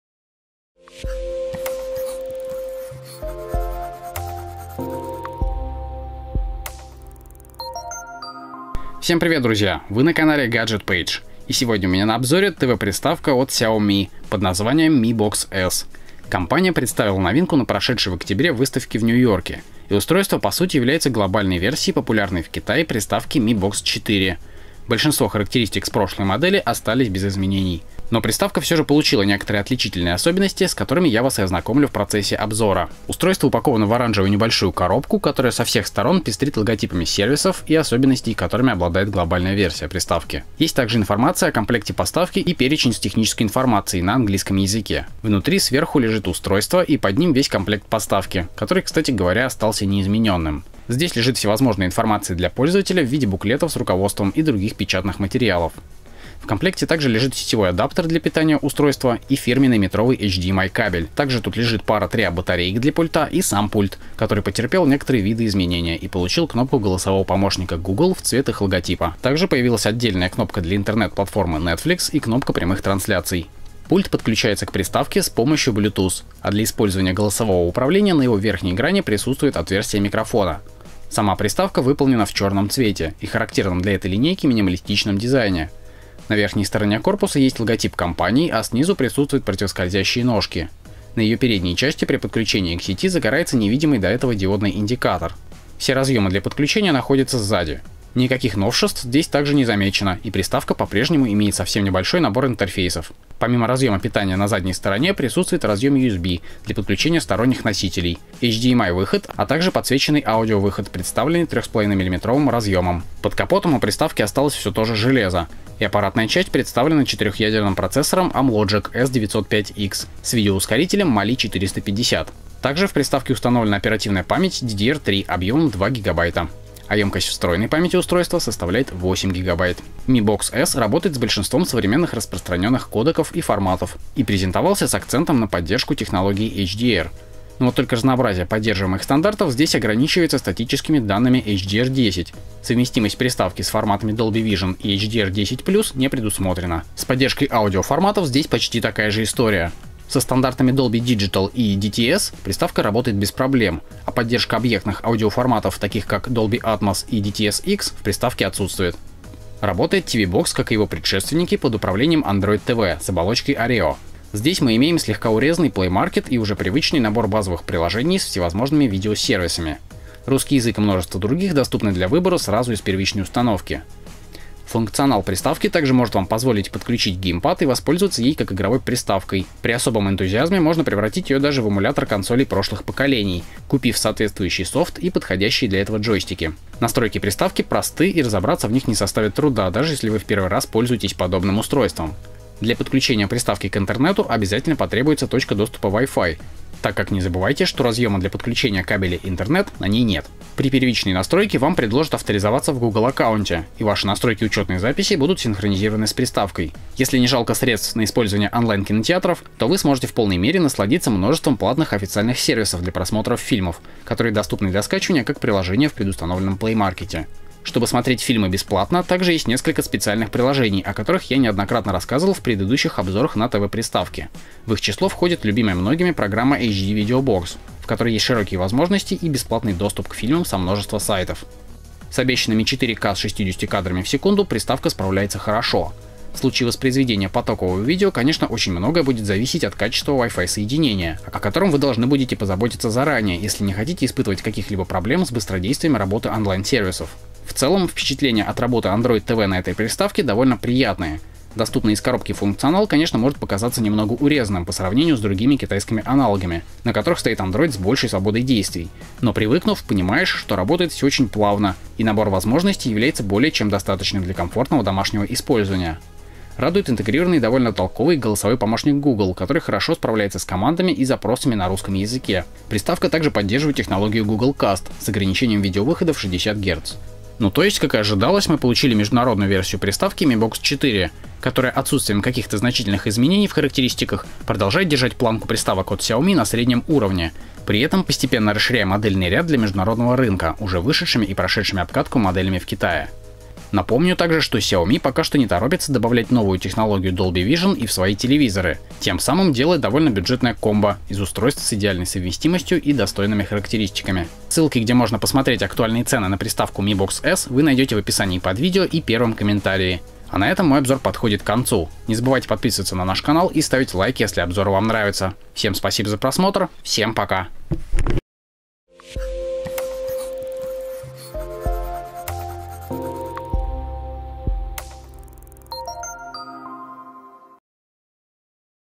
Mi Box S 4K - новая ТВ приставка от Xiaomi, международная версия с поддержкой Google Assistant
В этом видео мы покажем обзор на новую приставку  Xiaomi Mi Box S 4K